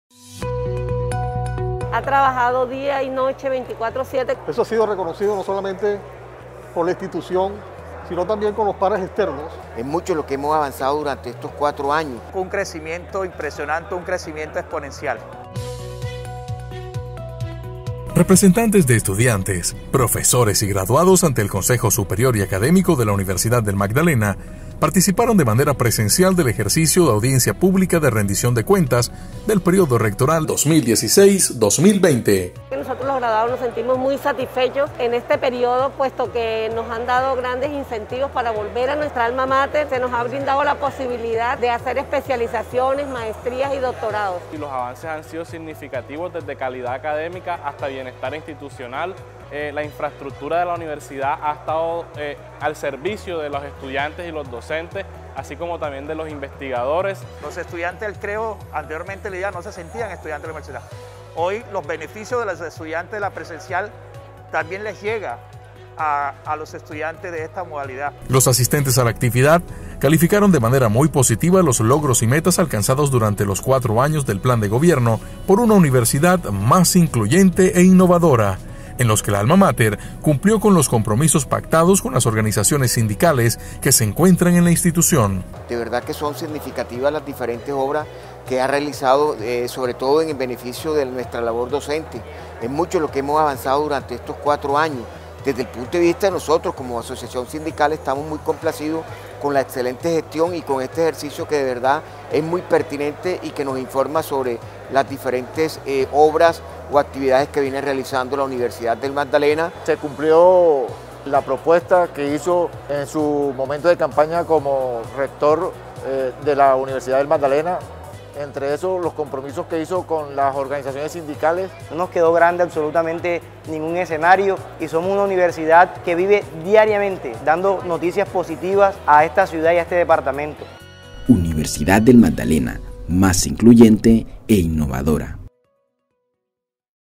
BA292_GESTIÓN DEL RECTOR PABLO VERA SALAZAR ES APLAUDIDA POR ESTAMENTOS UNIVERSITARIOS
Diferentes estamentos universitarios y líderes de agremiaciones sindicales que asistieron presencialmente al acto de Rendición de Cuentas, ponderaron la gestión del Doctor Pablo Vera Salazar al cierre de periodo rectoral. La comunidad universitaria y general participó de la jornada a través de la transmisión de Facebook Live, donde pudieron expresar sus inquietudes y preguntas sobre diferentes temas.